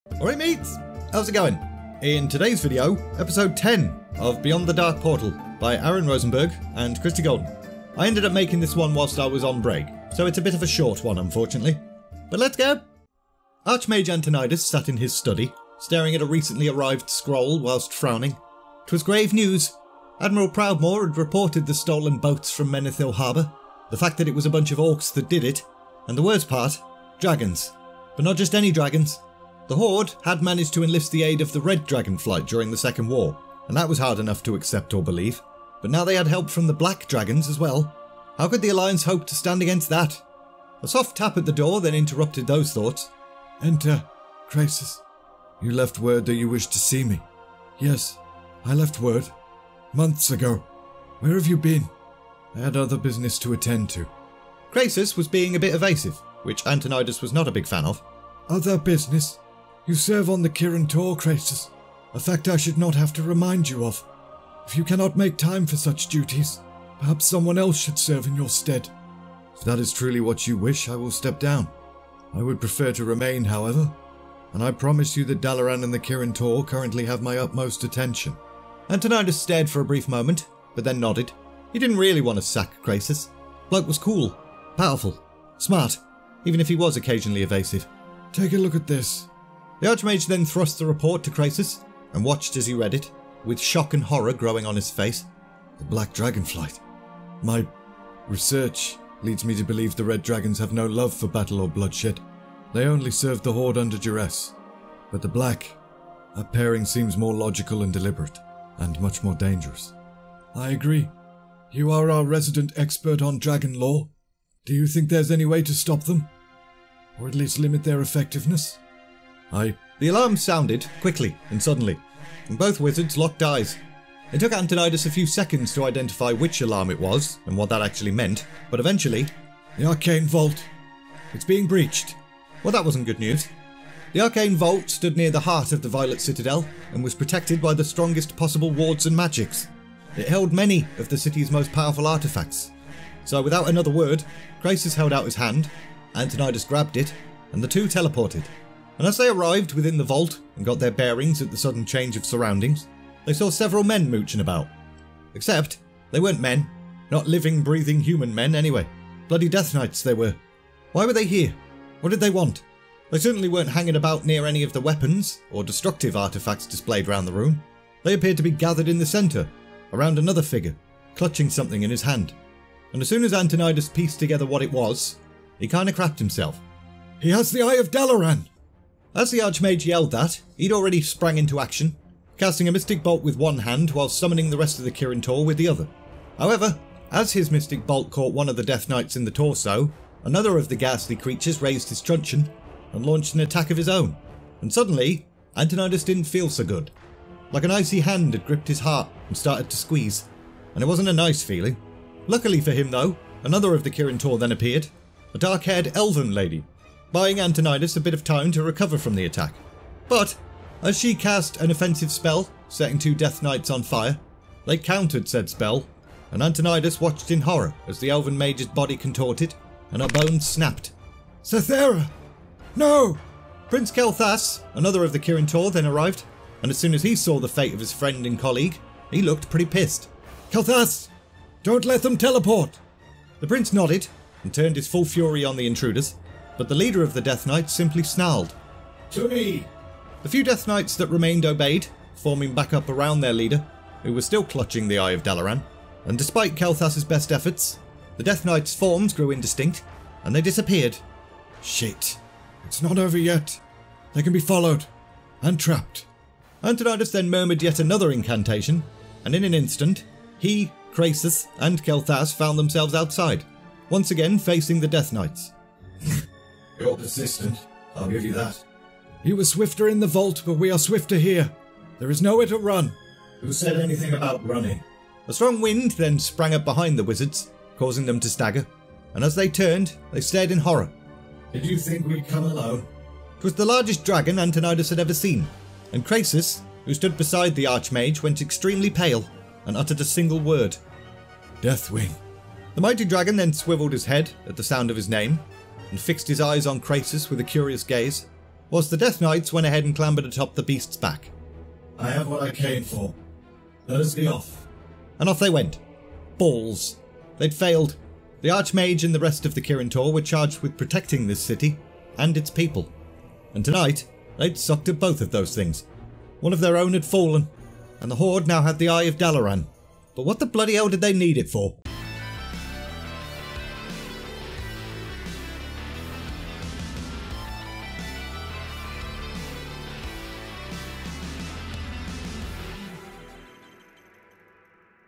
Beyond the Dark Portal [Warcraft Novel by Aaron Rosenberg & Christie Golden] - Episode Ten
An animated / audiobook adaptation of Chapter 11 from the World of Warcraft novel 'Beyond the Dark Portal' by Aaron Rosenberg & Christie Golden.

Short episode this week - Antonidas learns that the Black Dragonflight are now aiding the Horde.  But before he and Krasus can discuss any potential solutions to that problem; the arcane vault is breached!